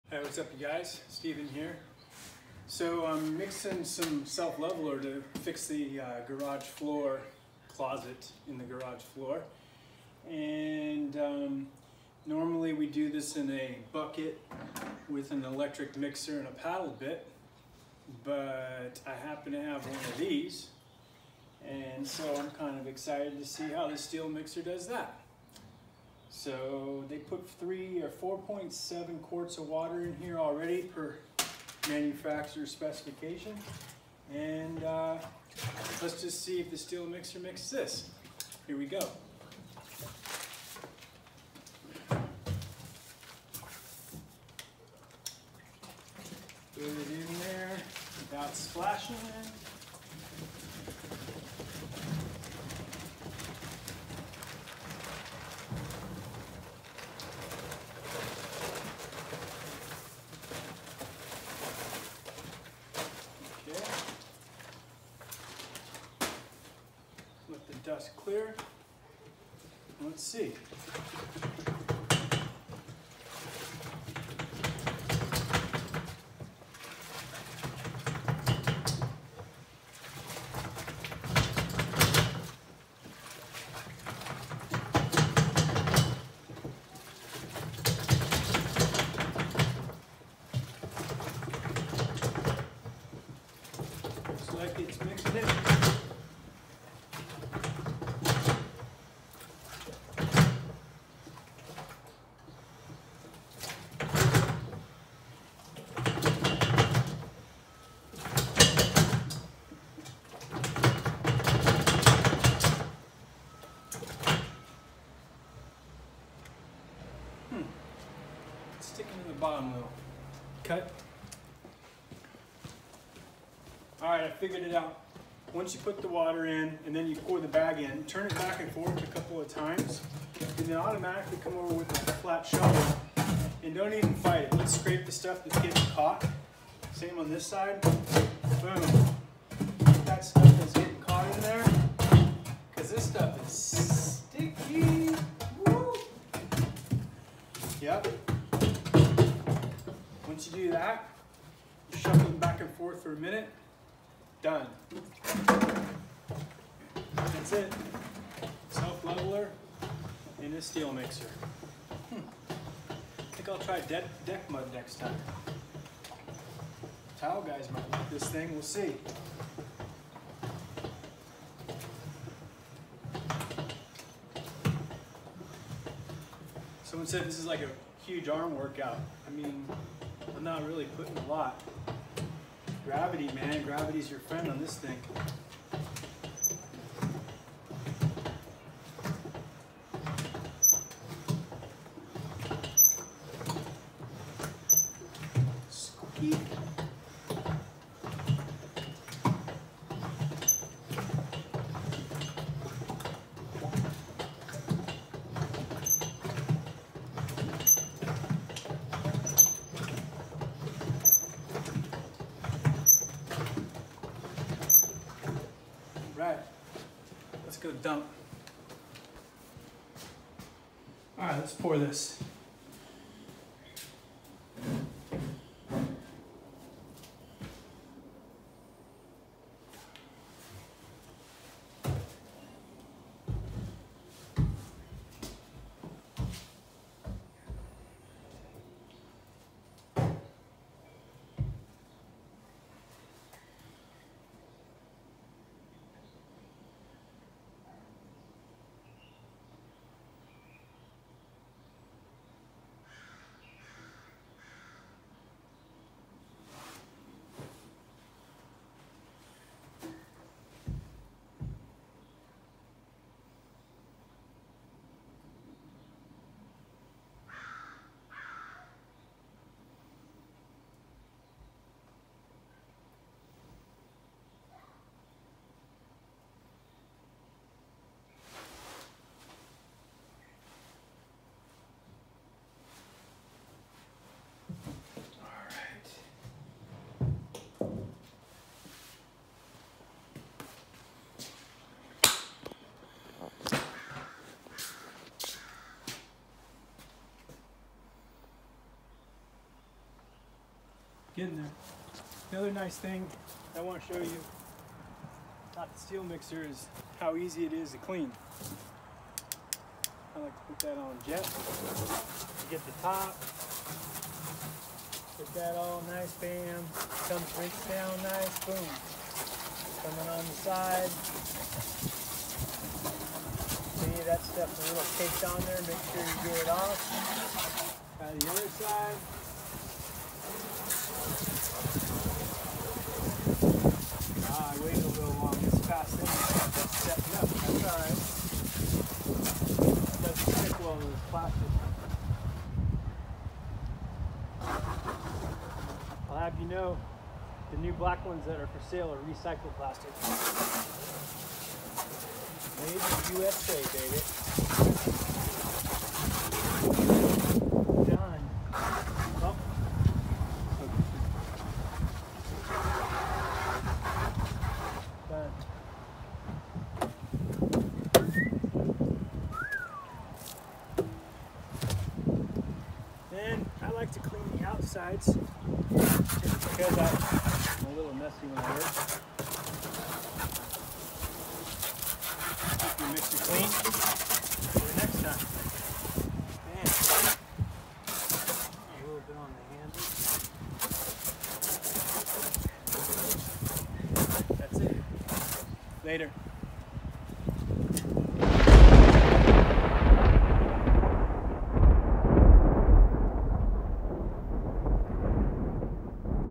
Mixing self leveling mortar in a SteeleMixer
The Steele Mixer can mix self leveling mortar as well as concrete!